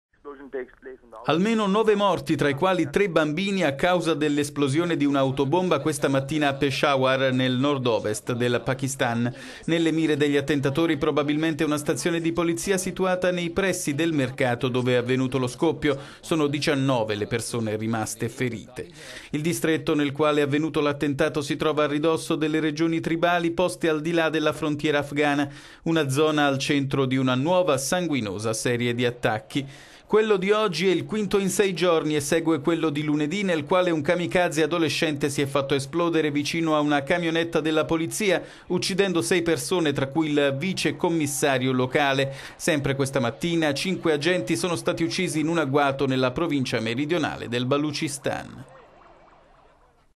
Pakistan: nove morti in esplosione al mercato
Almeno nove morti, tra i quali tre bambini, a causa dell'esplosione di un'autobomba mercoledì mattina a Peshawar, nel nordovest del Pakistan. Nelle mire degli attentatori probabilmente una stazione di polizia, situata nei pressi del mercato dove è avvenuto lo scoppio. Sono 19 le persone rimaste ferite.

Il distretto nel quale è avvenuto l'attentato si trova a ridosso delle regioni tribali poste al di là della frontiera afghana. Una zona al centro di una nuova sanguinosa serie di attacchi.